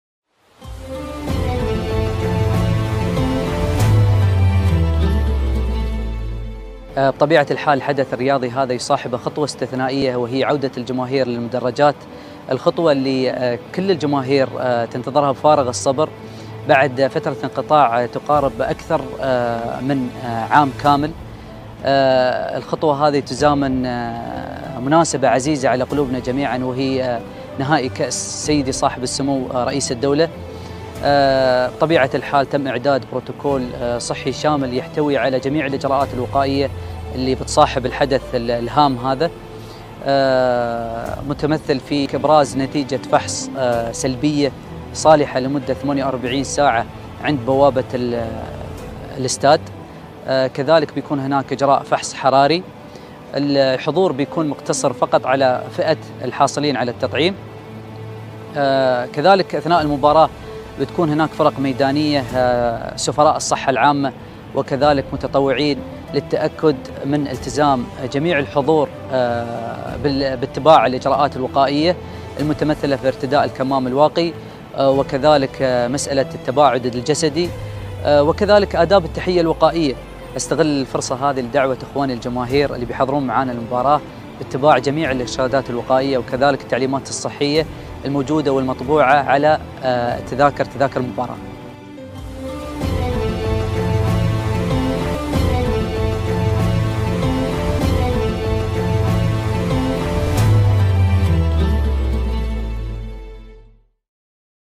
نهائي كأس رئيس الدولة
يوصي د. فيصل مصلح الأحبابي رئيس قسم برامج الأمراض المعدية في مركز أبوظبي للصحة العامة الجماهير بالإلتزام بالإجراءات الاحترازية والوقائية في نهائي كأس رئيس الدولة 🏆